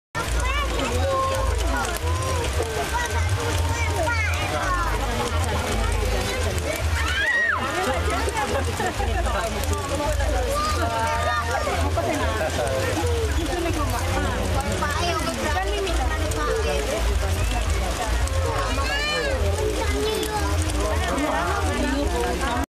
Koleksi Hewan KBS, Monyet Suka Hujan hujan 8
Jalan-jalan ke Kebun Binatang Surabaya